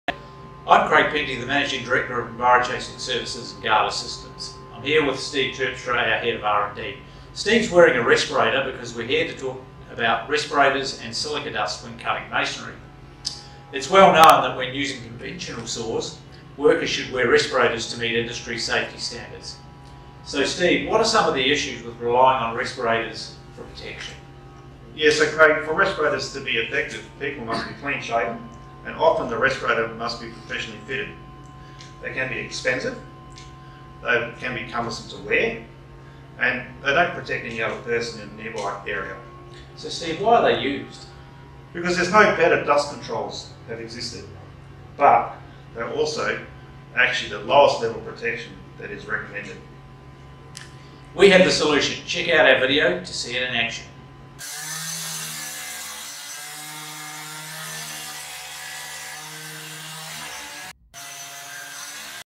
Respirators and Silica Dust
Steve Terpstra and I discussing some of the issues when relying on respirators for silica dust protection.